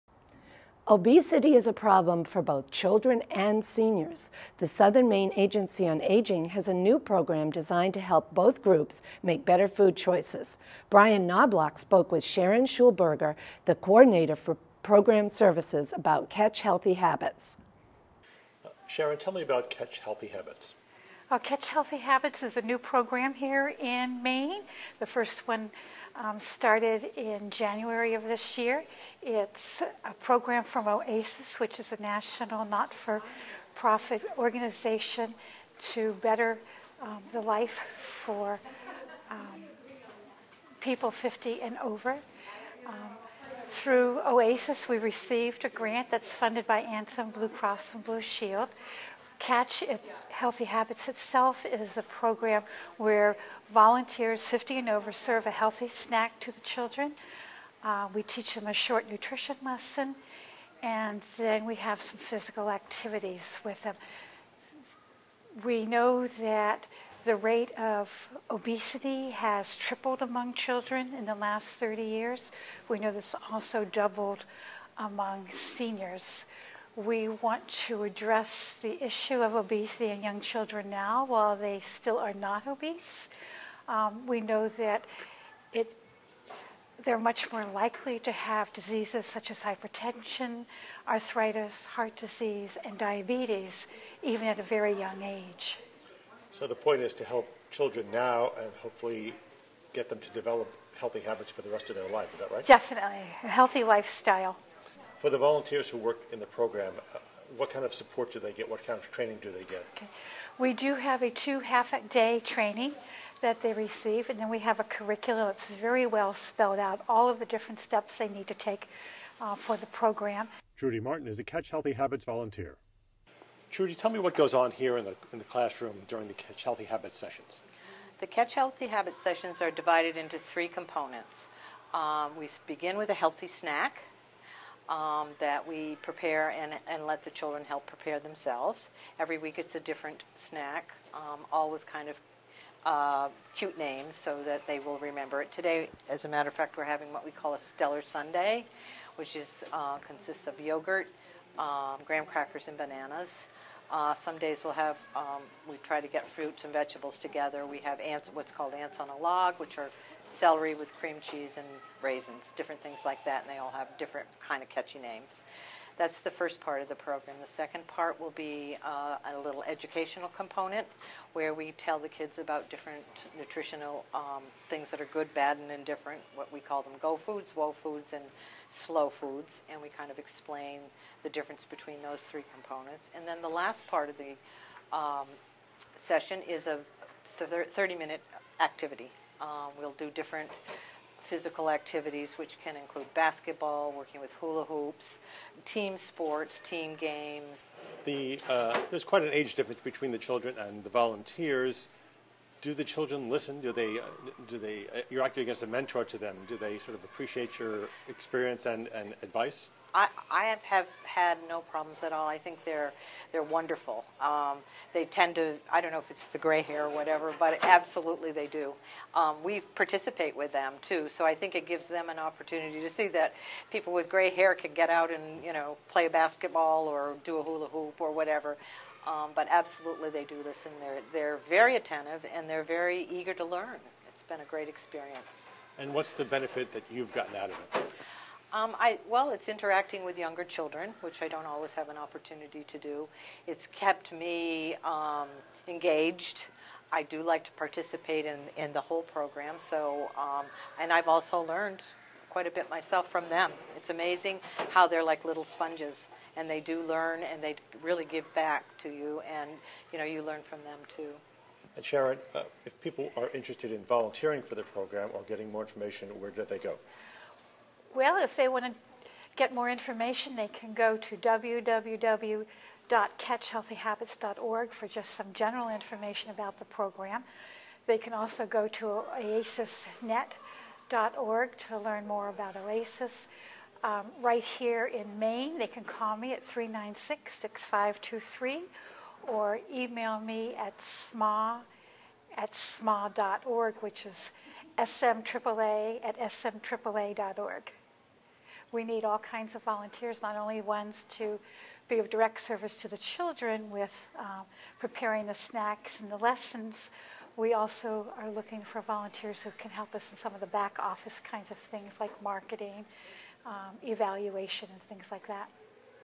Southern Maine Agency on Aging program to help children and seniors eat better.mov
The Southern Maine Agency on Aging Coordinator for Program Services, Sharon Schulberger, talks about their program to help children and seniors make  better food choices.